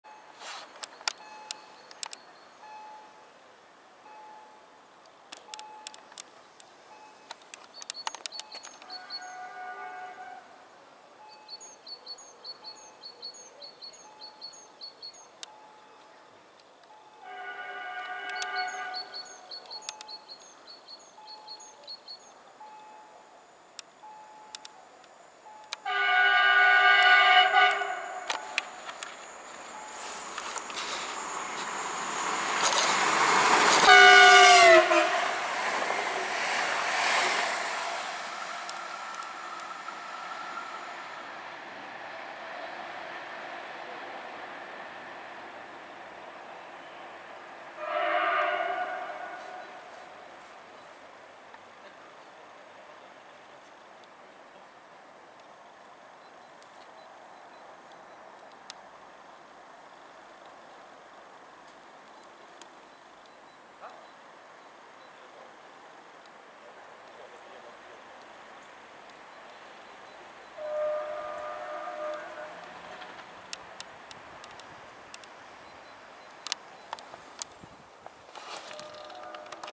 Koncertowy "Bajkowy" z Dopplerem :)
Przelot przez Międzyborów duetu Flirtów 3 ŁKA z ŁS "Bajkowy" do Łodzi Fabrycznej. Serdeczne  podziękowania i pozdrowienia dla kolegi za tą kanonadę i Doppler  :). Data nagrania: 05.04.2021